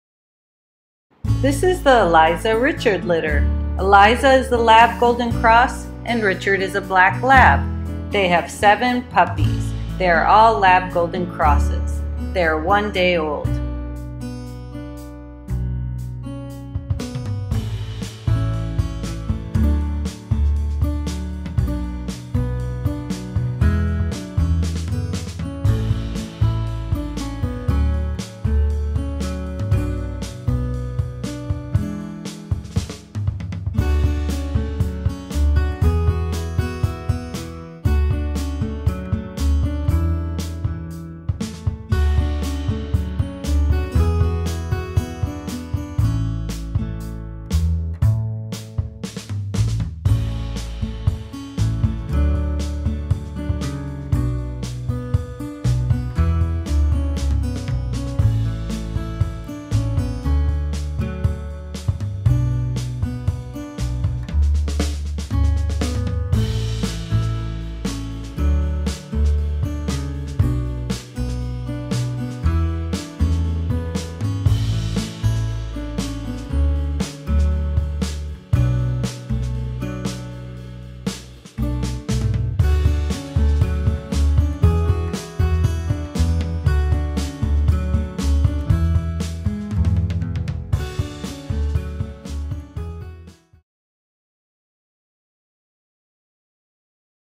04-09-2020 Eliza
Eliza x Richard litter.  2 black & 5 yellow Labrador/Golden Retriever Cross puppies.  1 day old.